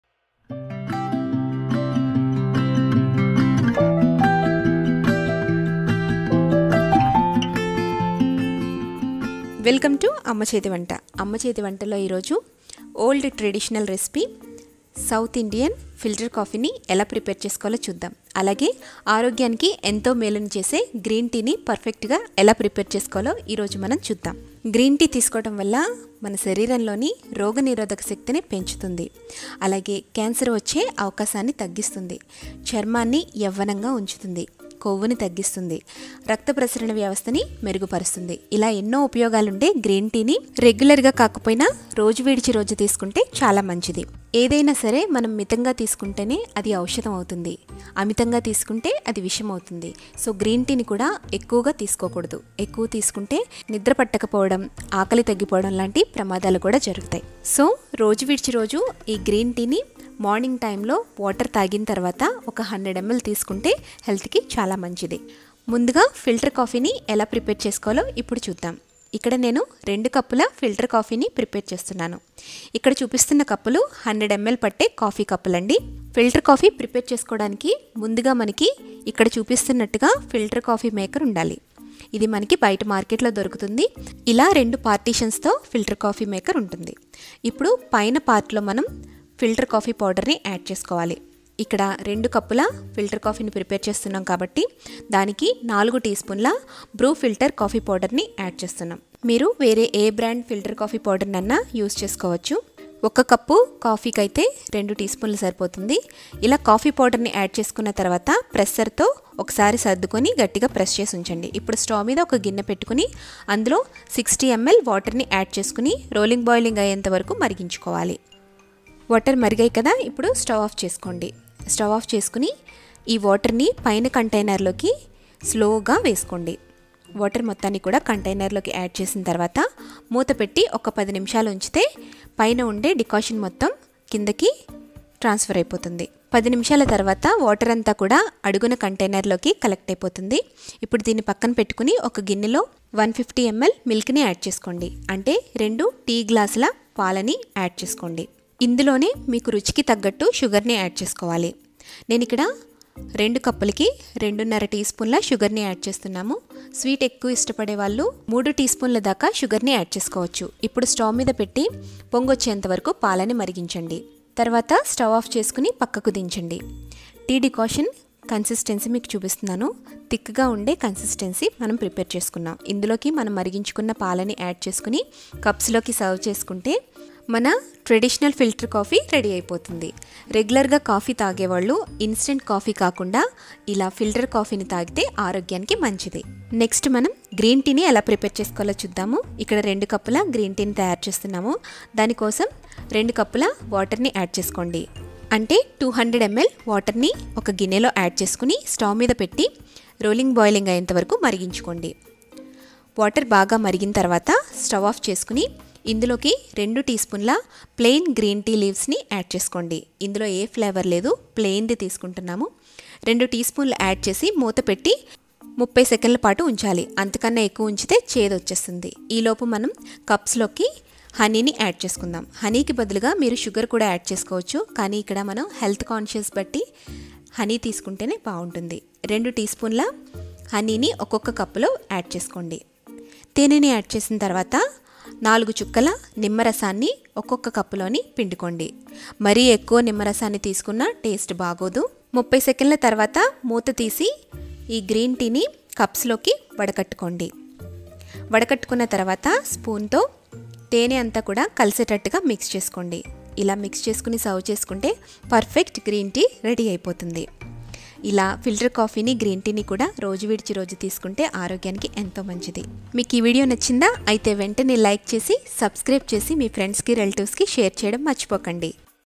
ఫిల్టర్ కాఫీ,గ్రీన్ టీ ఎలా చేయాలో చూడండి | How To Make Filter Coffee At Home | Green Tea In Telugu
Today in amma chethi Vanta we prepared a famous old traditional and healthy drink recipes South Indian Filter Coffee and Green Tea.Green tea is having best benefits for weight loss and other.It is best to drink filter Coffee instead of instant coffee daily. You can take these two drinks day after day in the morning time after drinking some water,daily 1time is enough.

Ingredients:

For 2 Cups Filter Coffee;
Water 60 ml
Filter Coffee Powder 4 tsp
Milk 150 ml
Sugar 2 tsp (according to taste)

For 2 Cups Green Tea
Water 2 Cups (100 ml each)
Green tea 2 tsp
Honey 4 tsp
Lemon juice 8 drops

Drink Varieties